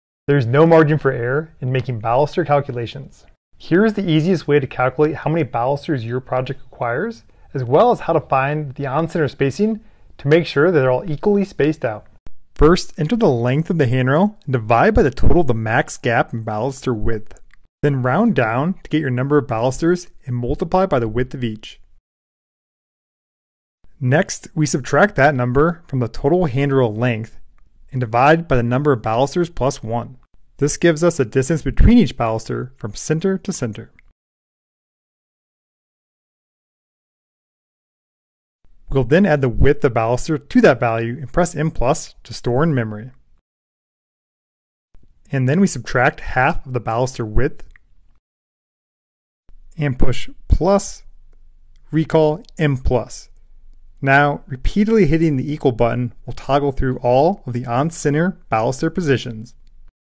How to calculate the number of balusters you need & find their on-center spacing
1) Enter the length of the handrail and divide by the total of the max gap and baluster width.

2) Round down to get your number of balusters and multiply by the width of each baluster.

3) Subtract that number from the total handrail length. Then divide by the number of balusters + 1. This gives you the distance between each baluster from center to center.
⚠️ make sure you add one to the number of balusters because you will have one more spacer than you do baluster.

4) Add width of baluster to that value and push M+ to store in memory

5) Subtract half of the baluster width

6) Push + ➡️ RCL ➡️ M+. Then press the ＝ button repeatedly to toggle through all of your on-center baluster positions.